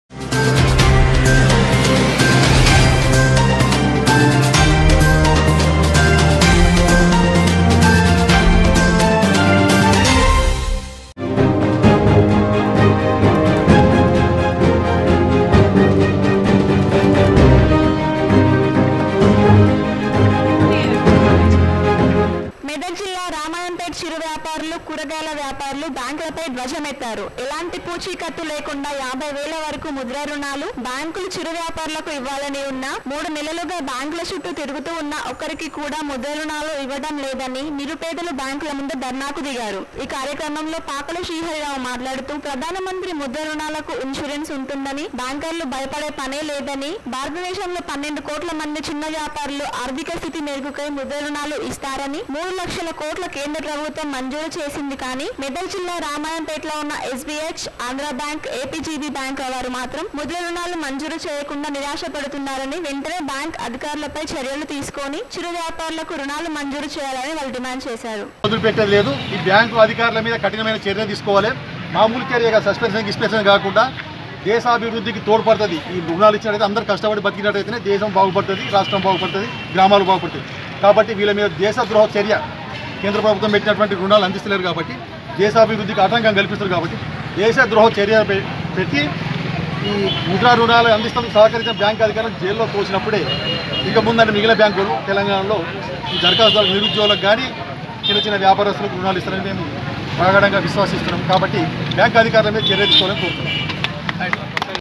మెదక్ జిల్లాలో చిరువ్యాపారులు ముద్ర రుణాల పై ధ్వజం | 28-06-2018 | C2 TELANGANA
C2 TELANGANA